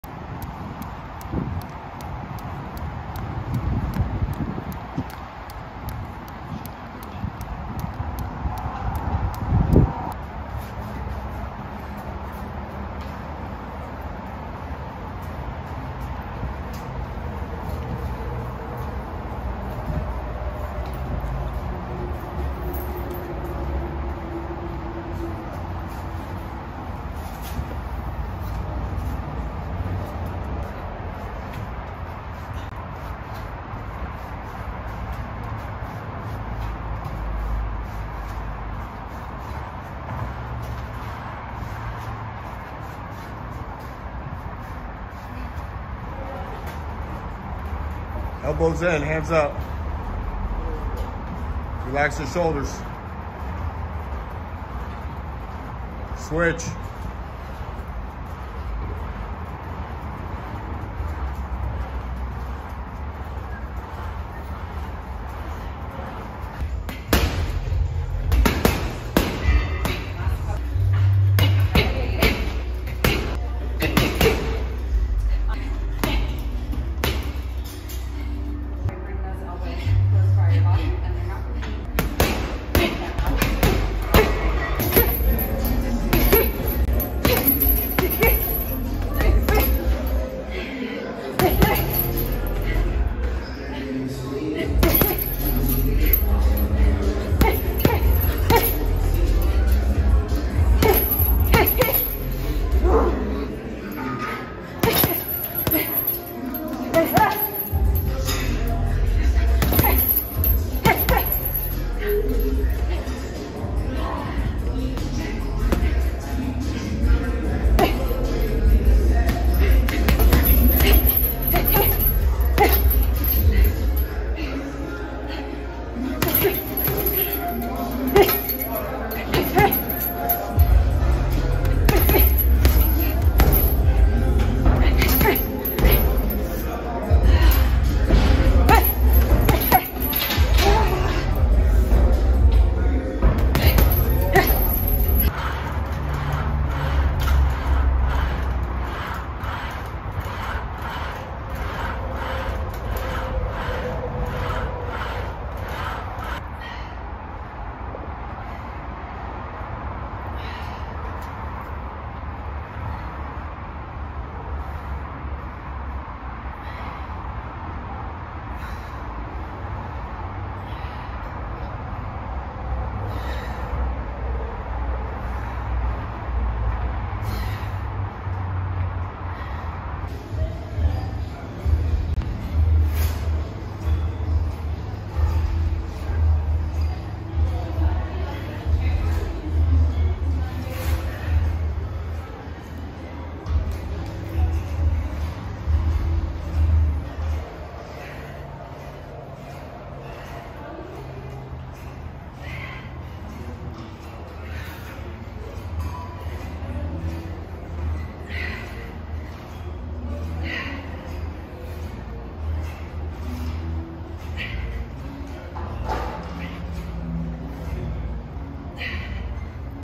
Hayley “Relentless” Jordon Training Routine
Hayley “Relentless” Jordan Training Routine

Jump Rope 3 Rounds
Shadow Box 3 Rounds
Footwork Drills 2 Rounds
Heavy Bag 3 Rounds
Aqua Bag 3 Rounds
Push-Ups (Med Ball)
Ab Work